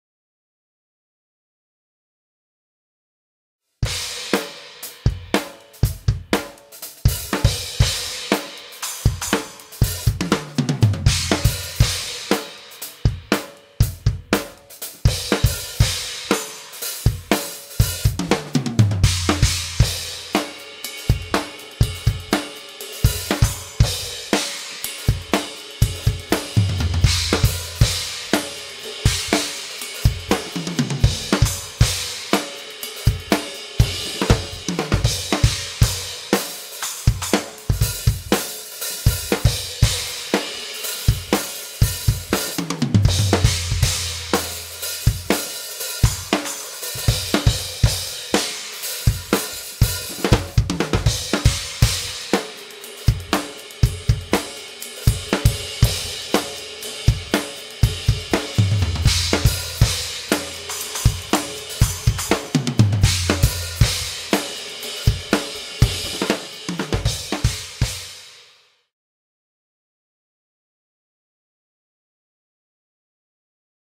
Supernatural Relic Series
The Relic Series projects itself dynamically into any drummer's arsenal and is the true definition of Supernaturals Cymbals. The most versatile of all the lines, Relic can crossover from Rock to Jazz and from Funk to Punk. The raw, natural coloring contrasts with the deep uneven lathing making this cymbal line a true "Treasure to be Found". Relic is formulated to give drummers of all styles the confidence to play for the music with crisp, dark crashes, defined and substantial rides and Hi-Hats you can really "lay into a groove with".